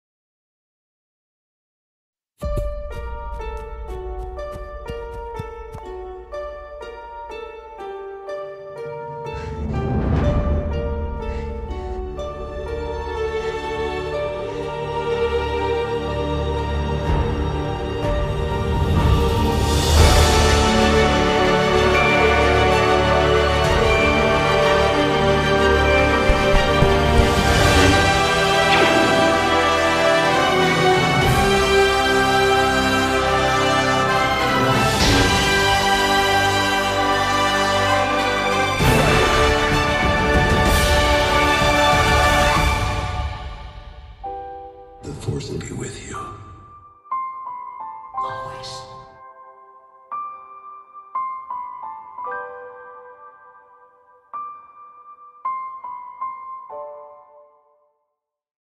REY: The Rise of Skywalker Cosplay | A Short Star Wars Fan Film
A long time ago, in a galaxy far, far away... I hand-made another Rey cosplay, this time her costume from The Rise of Skywalker! I was so chuffed with how this cosplay turned out, and I wanted to celebrate it by recreating a few scenes from the latest episode in the Star Wars saga.

Massive thanks to my sister and my mum for helping me shoot this one!